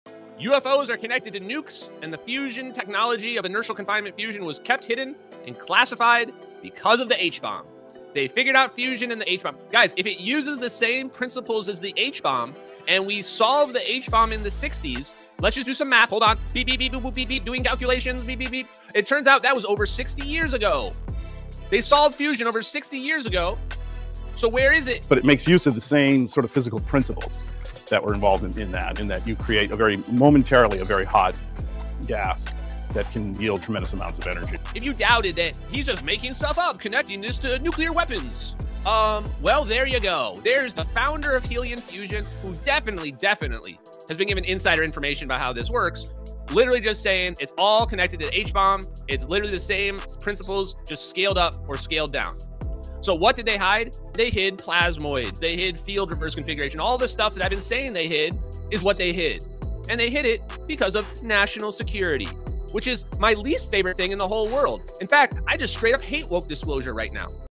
Did They Crack Fusion 60 Years Ago?
Fusion energy isn’t new. In fact, the physics behind it powered the H-bomb in the 1960s. So why don’t we have limitless clean energy today?